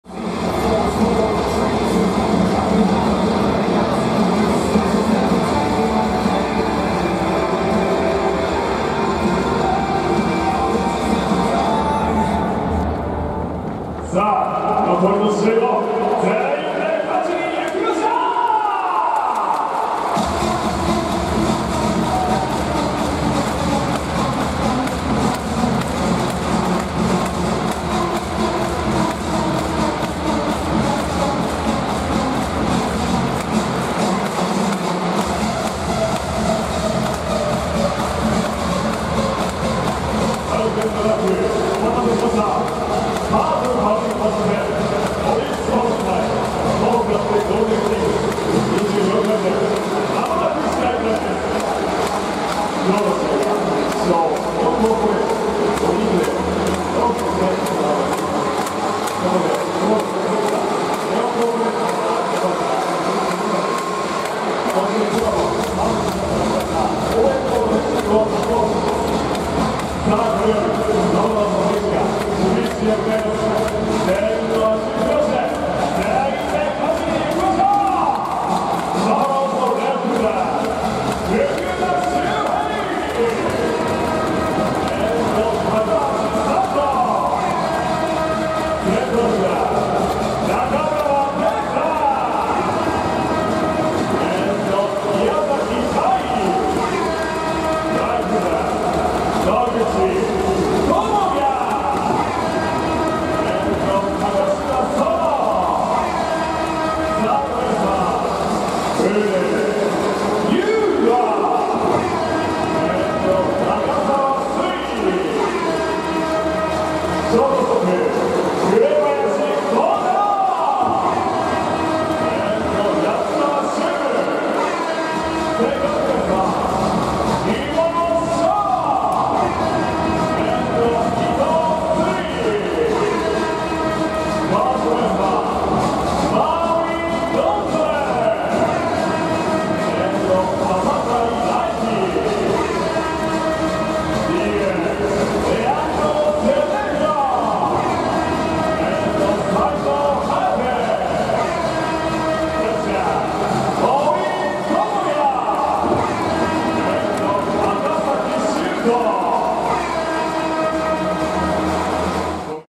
オリックス・バファローズ の選手が守備位置へ向かいます 全員で勝つ！
LF 福田 周平
CF 中川 圭太
RF 野口 智哉
5B 宗 佑磨
SS 紅林 弘太郎
4B 宜保 翔
3B マーウィン・ゴンザレス
DH レアンドロ・セデーニョ
C  森 友哉

バファローベル